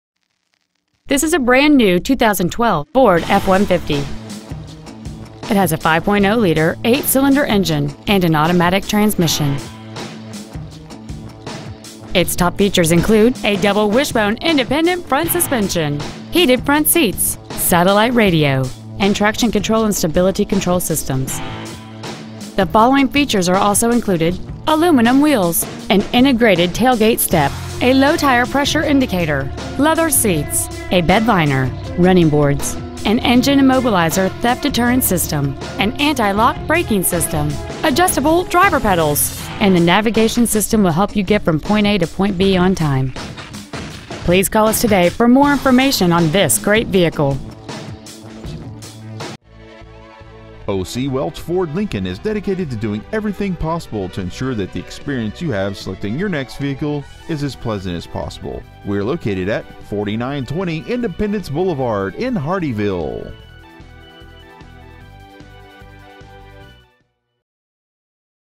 2012 FORD F-150 Hardeeville SC
Year : 2012
 Make : FORD
 Model : F-150
 Engine : 5.0L V8 32V MPFI DOHC FLEXIBLE FUEL
 Trans . : 6-SPEED AUTOMATIC
 Exterior : RACE RED
 Miles : 30

 Stock : 00T23521